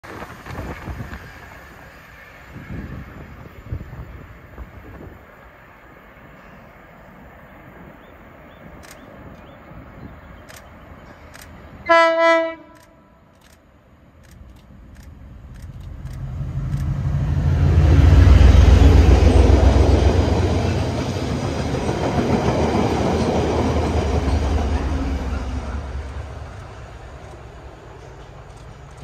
6Z95 wool winfrith sidings - Crewe coal sidings 68017 + 68016 24/03/22
68016 + 68017 working down to wool winfrith sidings to clear out the nuclear flasks wagons down there only assuming the site it closing down